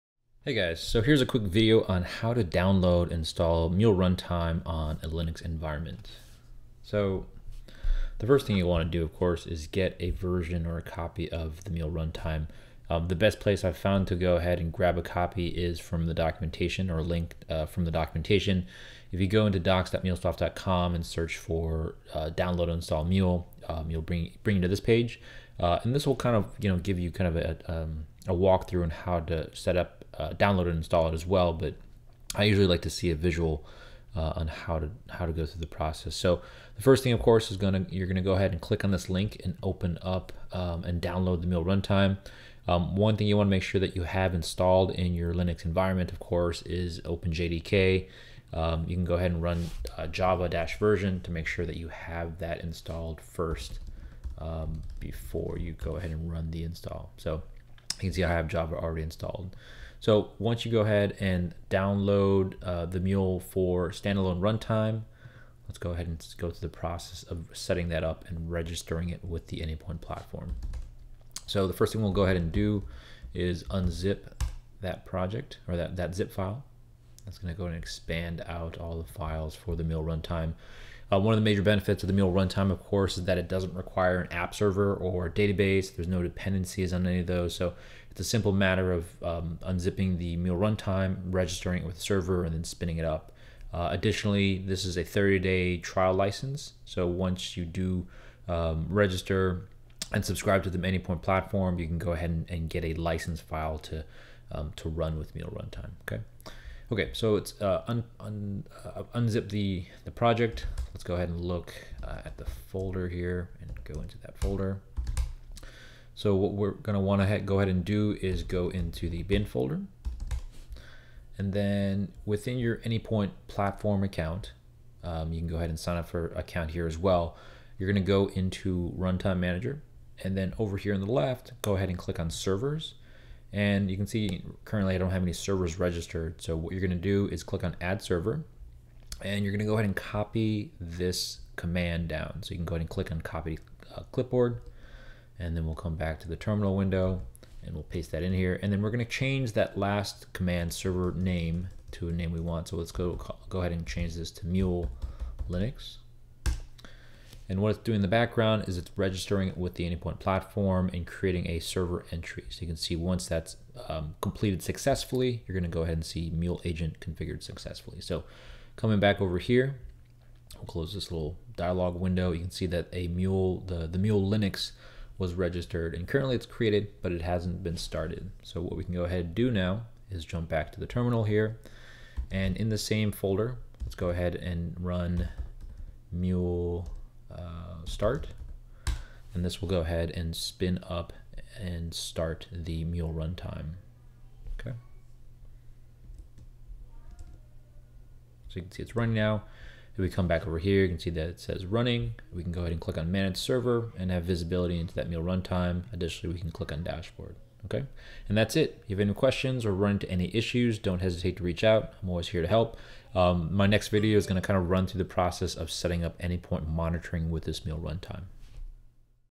How To - Download & Install Mule Runtime (4.x) - Linux
Quick video on where to download the Mule Runtime (4.x) and how to and install and register it with the Anypoint Runtime Manager on Linux.